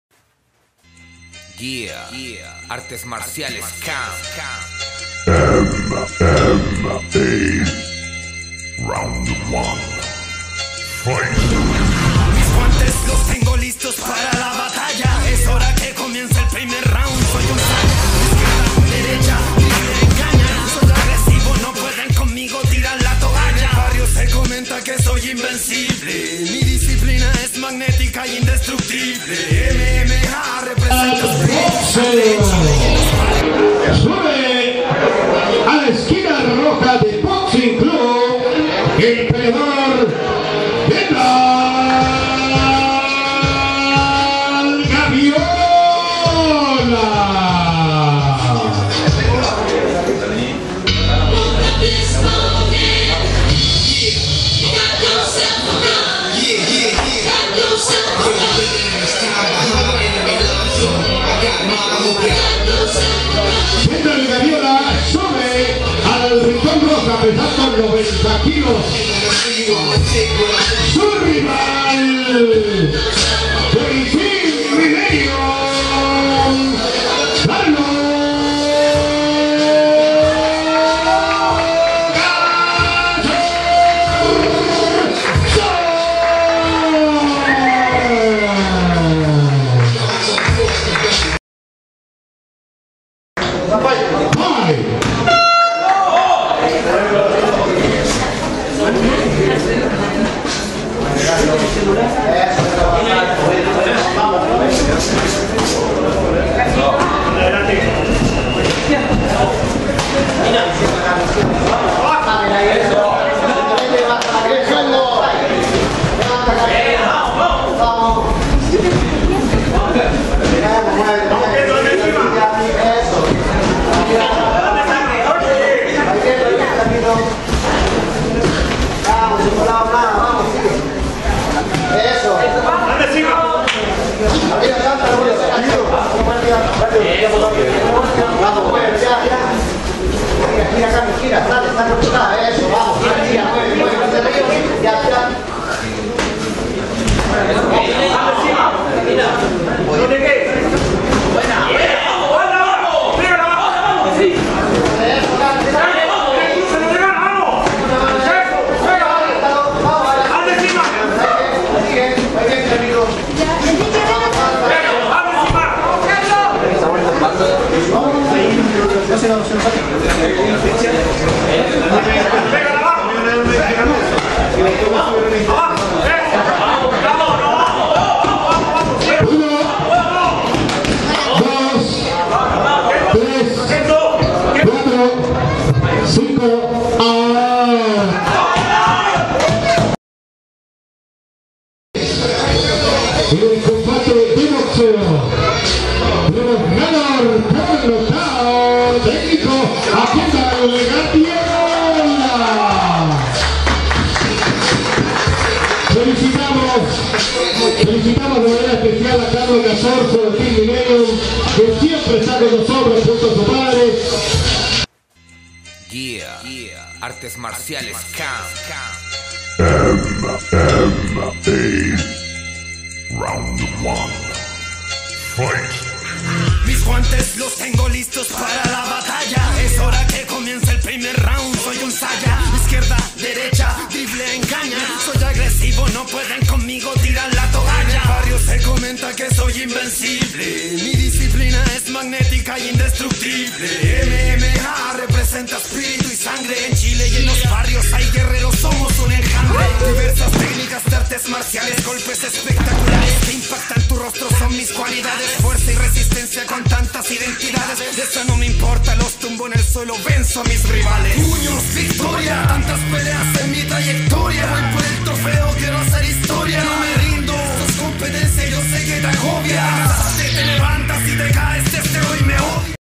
TORNEO CAM 11 Combate Nº14 Kendall Gaviola V/S Carlo Casorzo
Combate  Nº 14
CAM 11 Repechaje
Santiago ---Chile    20 Oct. 2012
modalidad BOXEO

Kendall Gaviola. CLUBBOXING
V / S
Carlo Casorzo. CLUB MILLENIUM SANTANDER

Ganador decision Abandono K.O.T.
Kendall Gaviola